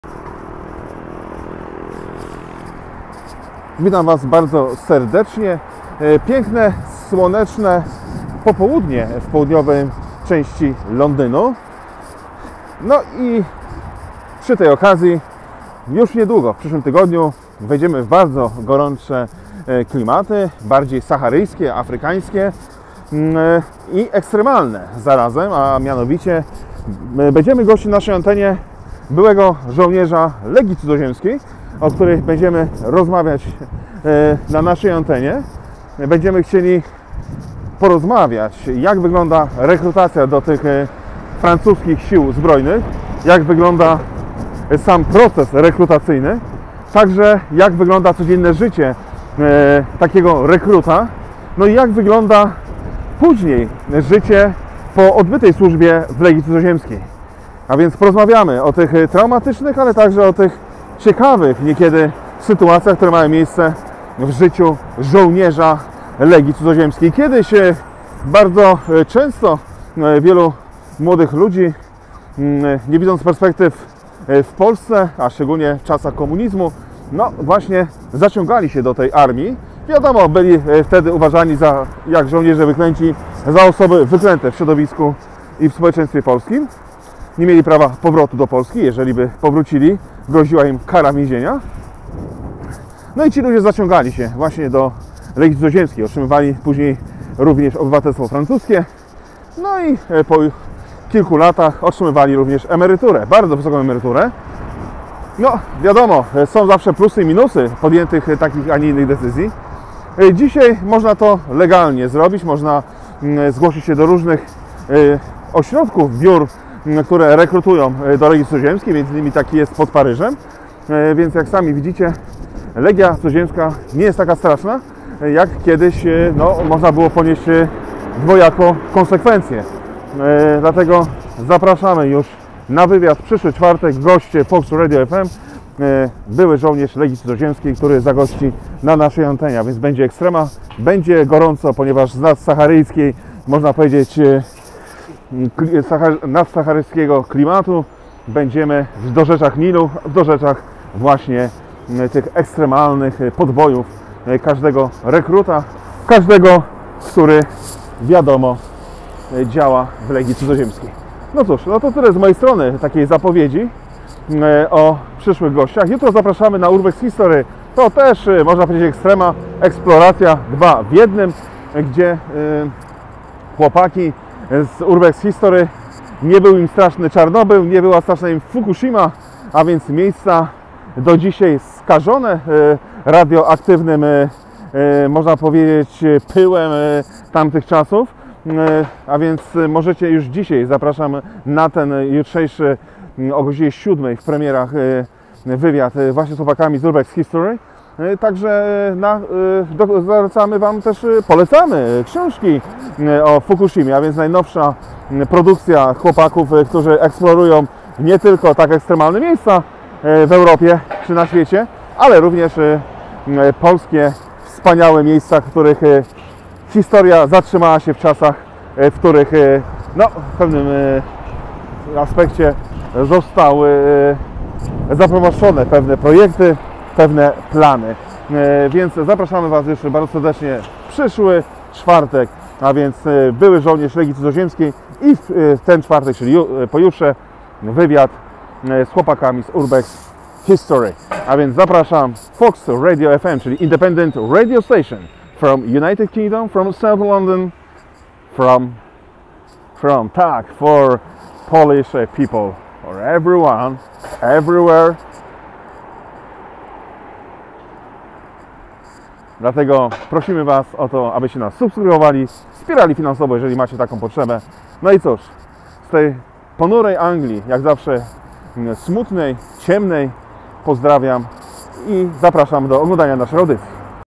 LEGIA CUDZOZIEMSKA FM-zapowiedź#19/01/2021
Już wkrótce na Naszej antenie będziemy gościć byłego żołnierza LEGII CUDZOZIEMSKIEJ.
ZAPRASZAMY.